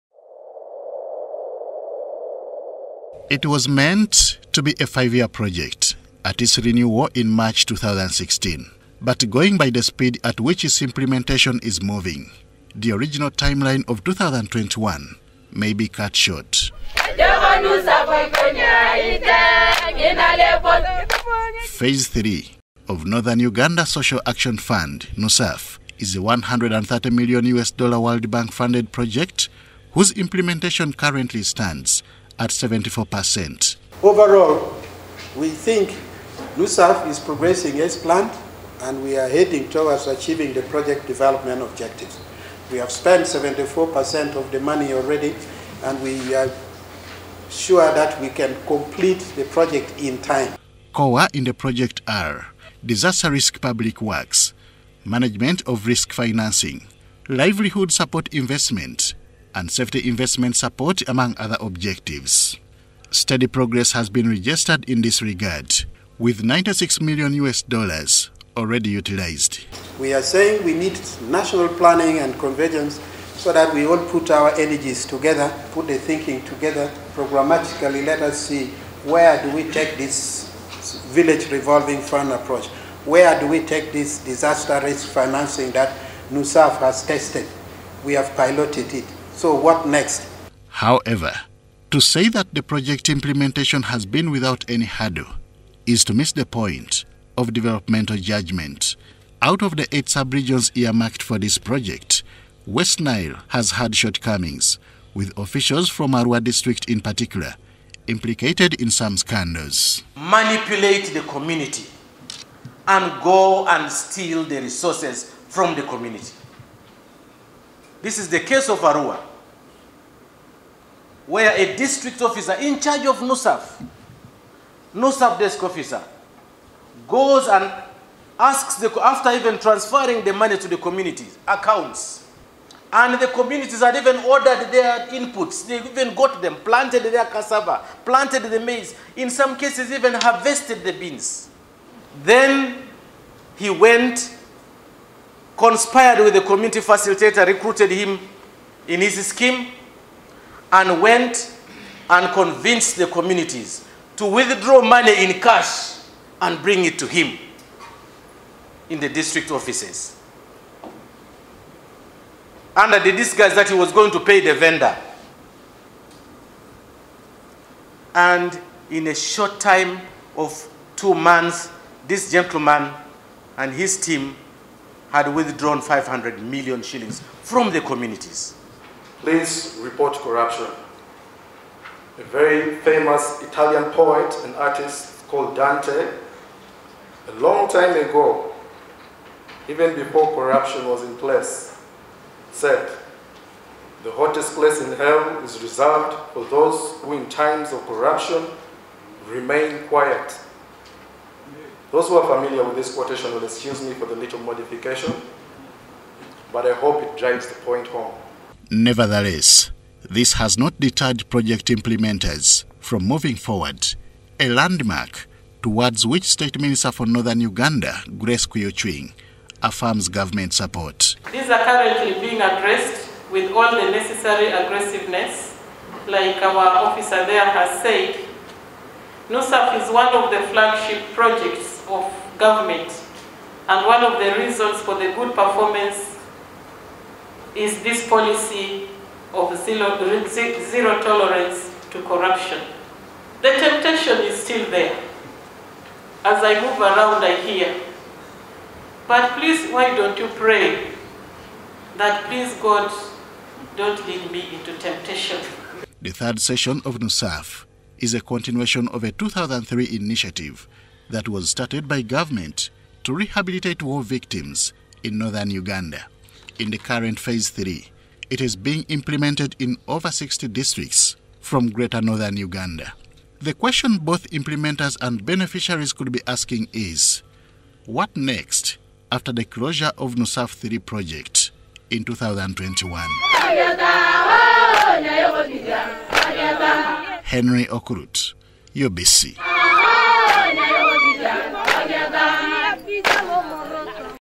NUSAF PROJECT PHASE III REGISTERS 74% PROGRESS
Government has registered significant progress in the third phase of the Northern Uganda Social Action Fund – NUSAF with seventy percent of the project already implemented.
The Director NUSAF at Office of the Prime Minister Dr. Robert Limlim however decries graft tendencies by some district officials leaving would be community beneficiaries cheated.
This was during the midterm review of the third session of NUSAF at an event officiated by State Minister for Northern Uganda Grace Kwiyucwiny in Kampala.